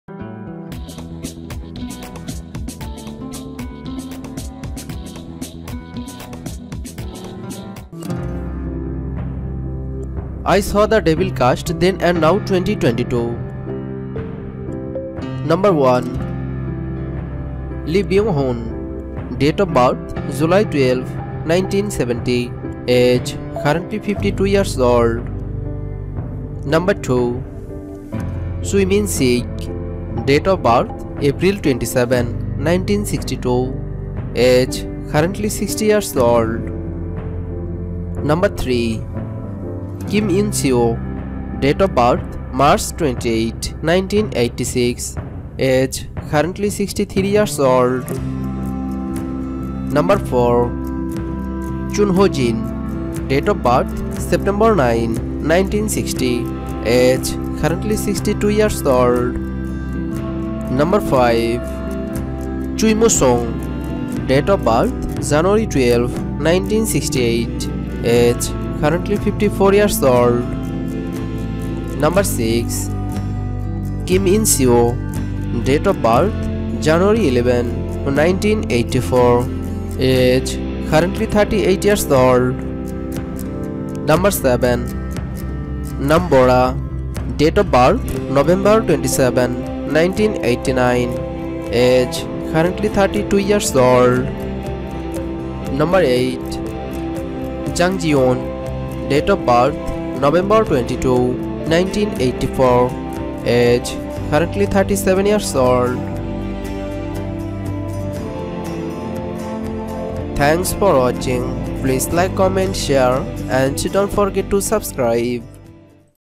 I Saw The Devil Cast Then And Now 2022
I Saw the Devil (Korean: 악마를 보았다) is a 2010 South Korean action thriller film[3] directed by Kim Jee-woon and written by Park Hoon-jung. Starring Lee Byung-hun and Choi Min-sik, the film follows NIS agent Kim Soo-hyun (Lee), who embarks on a quest of revenge when his fiancée is brutally murdered by the psychopathic serial killer Jang Kyung-chul (Choi). I Saw the Devil made its premiere in the United States at the 2011 Sundance Film Festival and had a limited U.S theatrical release.[4]

I Saw the Devil was Choi Min-sik's first major role since the changes to the Korean screen quota system.